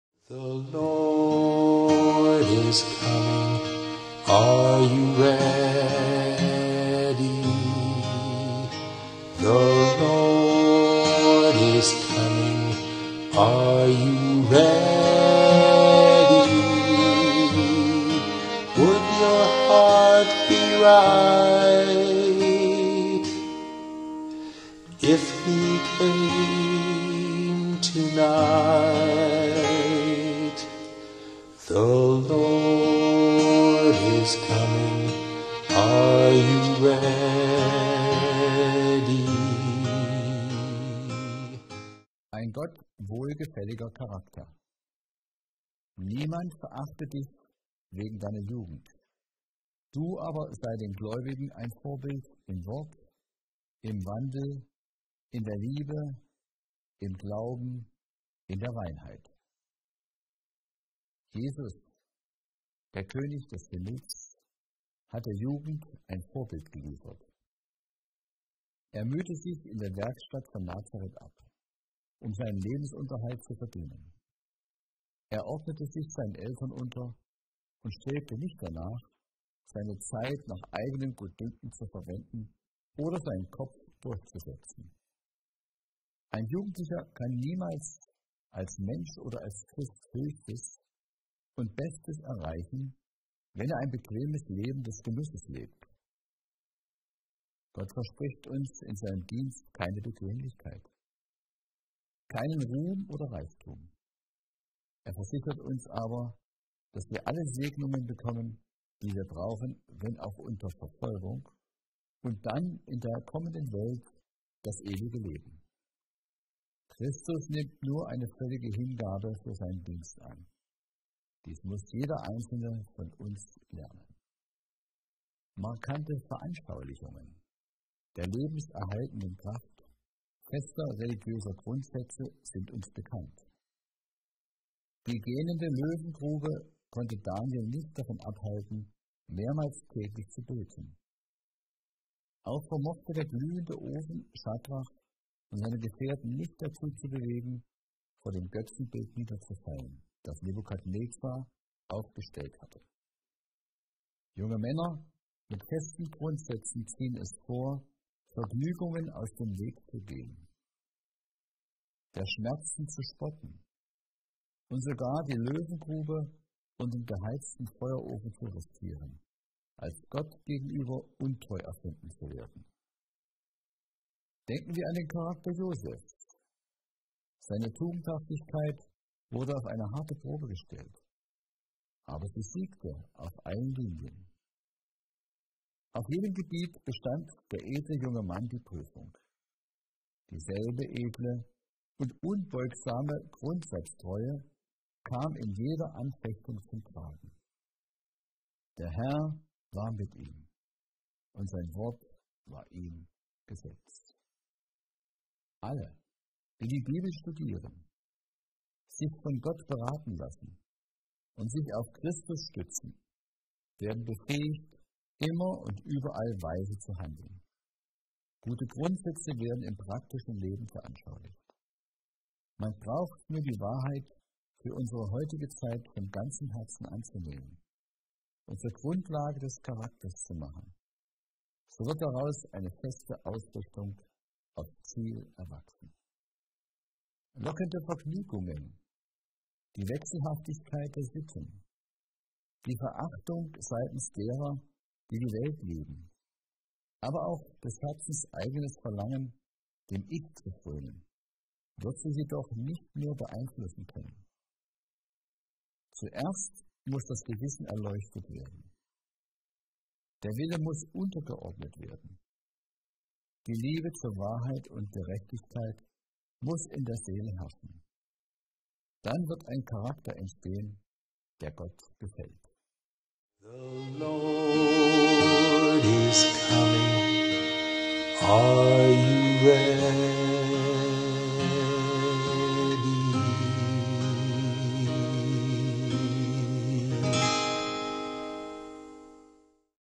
Maranatha - 16.03.2014 - Ein Gott wohlgefälliger Charakter
Motivation für den Tag
Junge Christen haben ein Vorbild. Die Bibel zeigt uns an Hand von Lebens beispiele wie junge Menschen den Angriffen Satans widerstanden haben und als Sieger hervorgegangen sind.